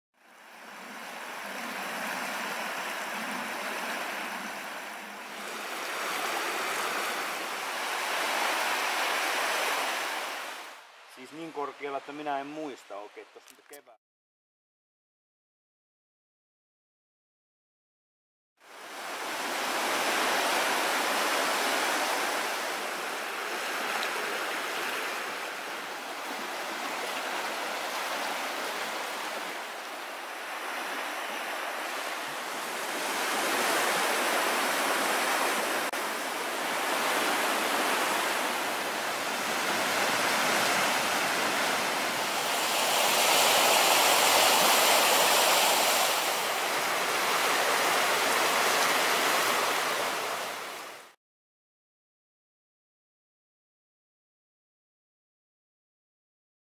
Kaivannonkoski talvella
Mäntyharjulla sijaitsevan Kaivannonkosken kuohuja maaliskuussa 2018.